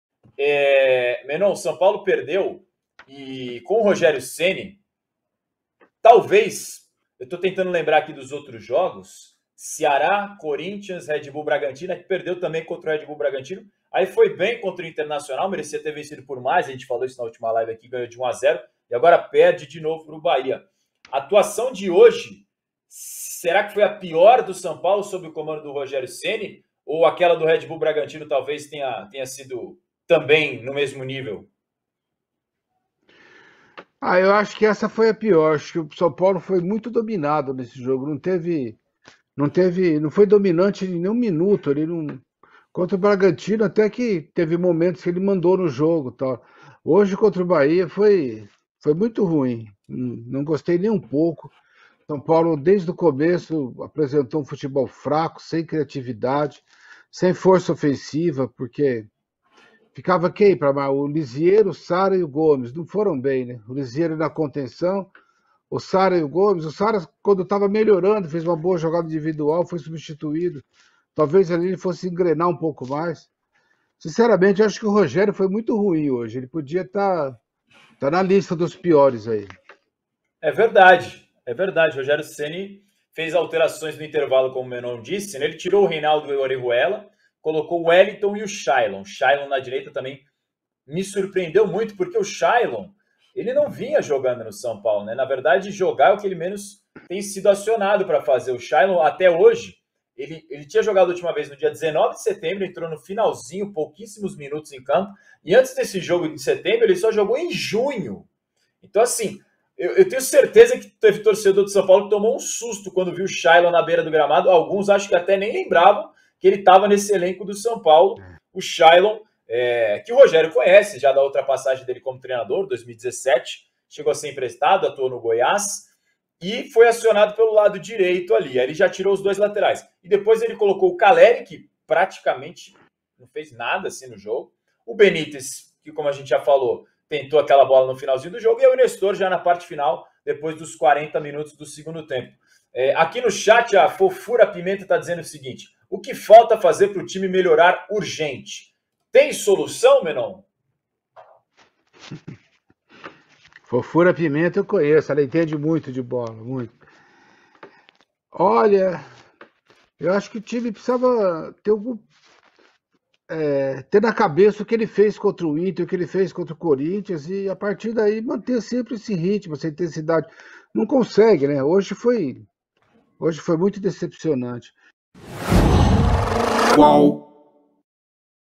O PIOR JOGO com CENI: São Paulo foi fraco, sem criatividade e não teve força ofensiva, diz Menon
Na Live do São Paulo UOL deste domingo (7), Marcelo Hazan e Menon analisaram o desempenho ruim do São Paulo na derrota de 1 a 0 para o Bahia na Fonte Nova. Para os comentaristas, foi o pior jogo da equipe paulista com Ceni no comando.